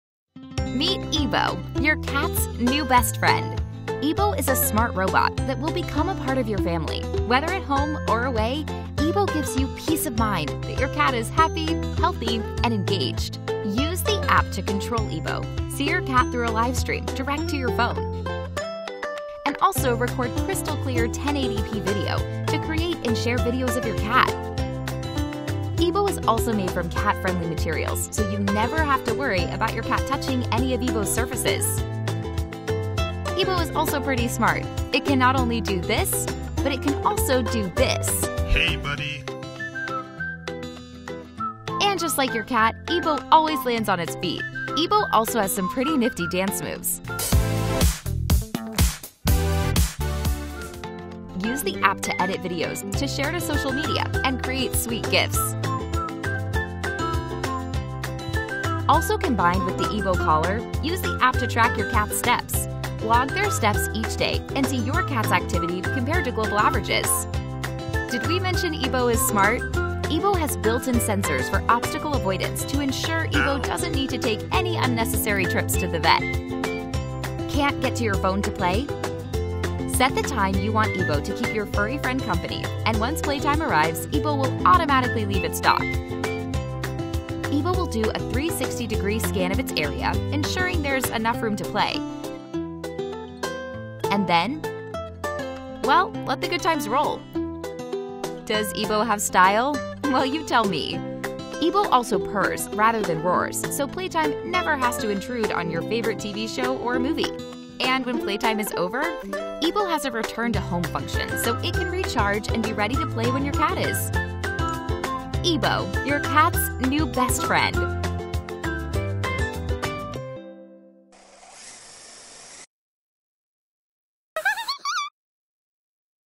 The best robot for cat companion.
the cats best friend, Love, Care, and Always Be There with Ebo, the Next Generation Cat Companion.